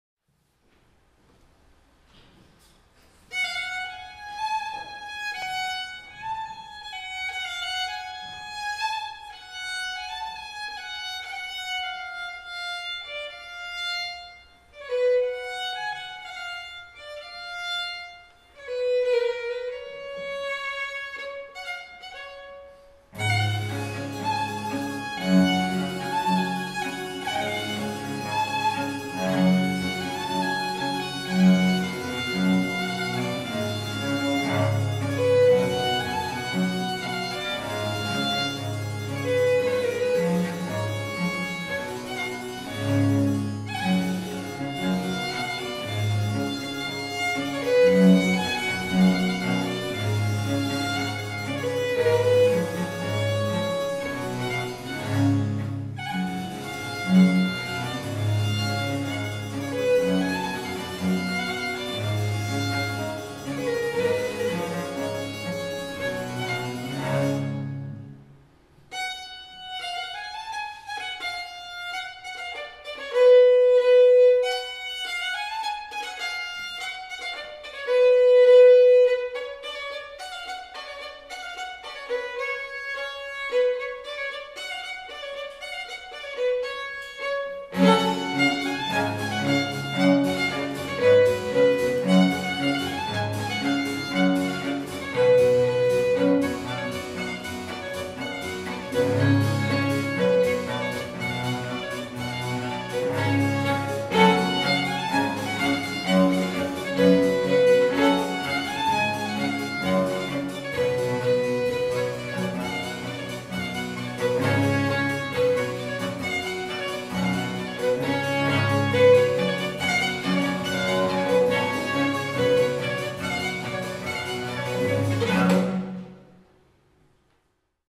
Dances from the Sf. Gheorghe manuscript (South-East Transilvania, 1757)
They have a strong oriental character, and according to an turkish music specialist (Constantin Raileanu), one is in makam hikaz and the other in makam nikriz